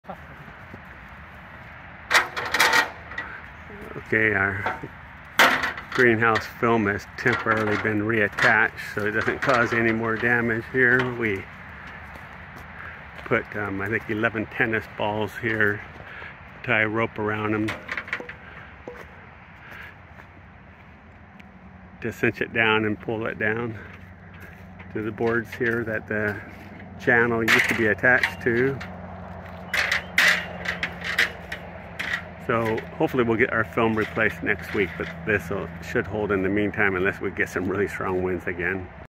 Temporary repair to High Tunnel
Recent severe storms have caused some delays with our high tunnel suffering significant damage Saturday night from winds reaching about 60 MPH.  We've purchased new film and related materials and arranged to have a contractor install it for us since we are too busy preparing for our fig cutting sale but they can't come until weather conditions improve, maybe next week (probably longer.  I decided that some temporary repair was needed as the film had blown over the top and was about to damage some trees on the other side of the high tunnel.  I had a bunch of tennis balls (toy for a pet) so I used those to help secure the film today when we got a break in the weather.

Back to office work on the shopping cart, don't have a good estimate yet of our sales date.